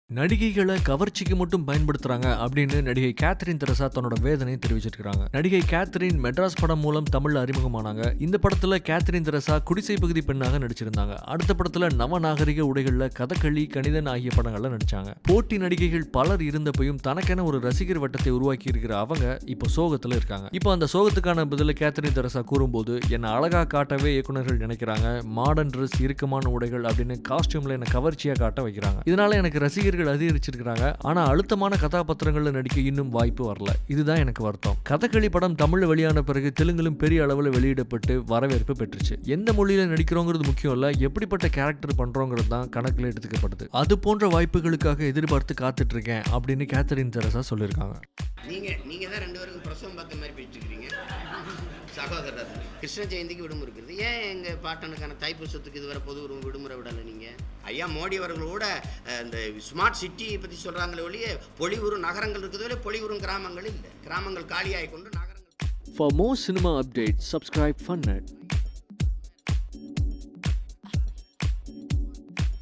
Catherine Teresa Angry at Directors - Kollywood Tamil Cinema Latest Hot news | Cine Flick
'Glamour isn't only my job" - Catherine Theresa Angry at Directors | Cine Flick